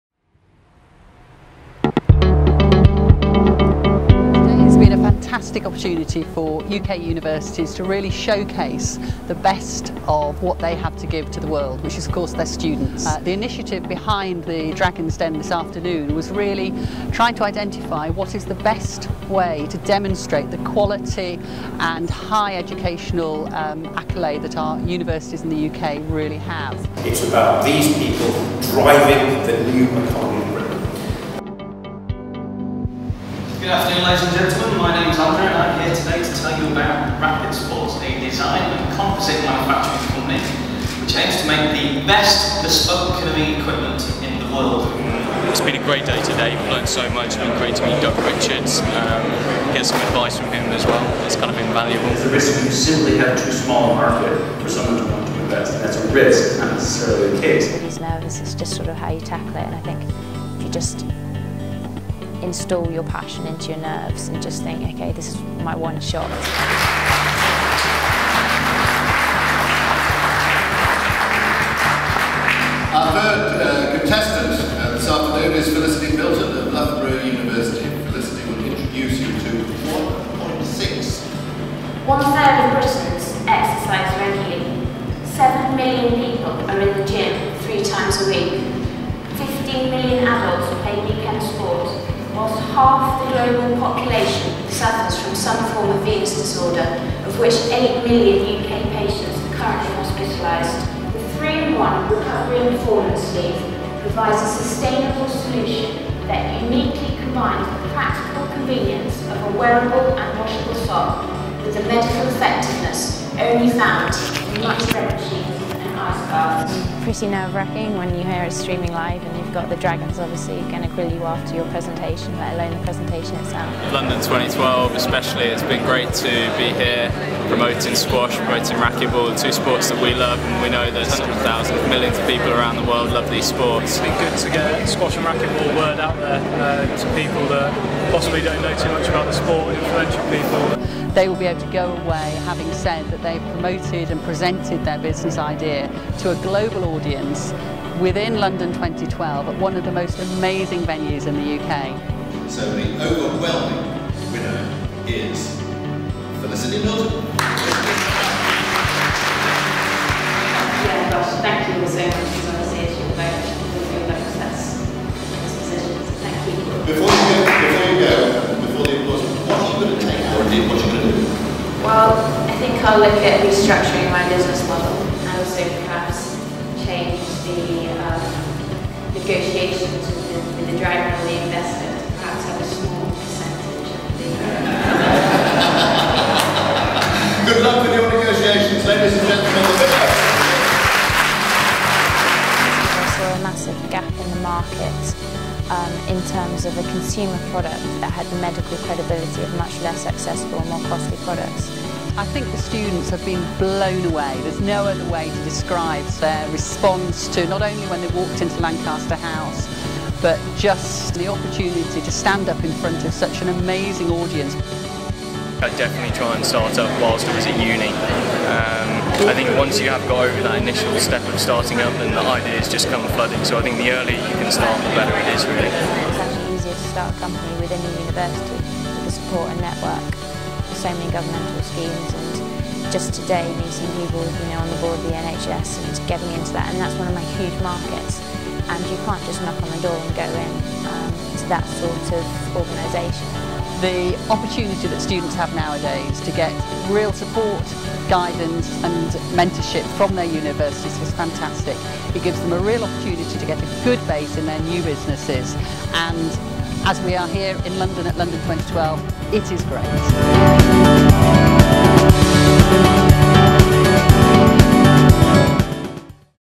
Dragons' Den at the Great Britain Campaign
The British Business Embassy and the Olympic International Business Legacy programme's Education Day, saw top student entrepreneurs pitch in the Dragons' Den. over 90 competitors from universities across the country battled it out, for a chance to be one of the four finalists who got to attend the coveted event in Lancaster House.

The Olympics-themed competition, conducted by National Centre for Entrepreneurship in Education (NCEE) with UKTI and the University of York, aims to support and promote entrepreneurship in Higher Education across the country.

Felicity Milton from Loughborough University was the overall winner with an 81% majority vote for 1point6, and her patented 3-in-1 recovery performance sleeve which enhances athletic performance, aids recovery and prevents injury.